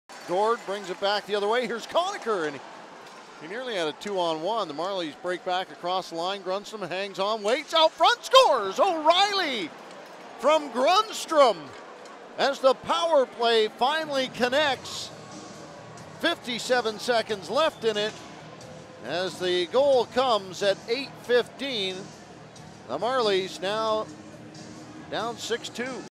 Cal O'Reilly Goal - May 6, 2017
Cal O'Reilly makes no mistake on a beautiful feed from Carl Grundstrom in the second period.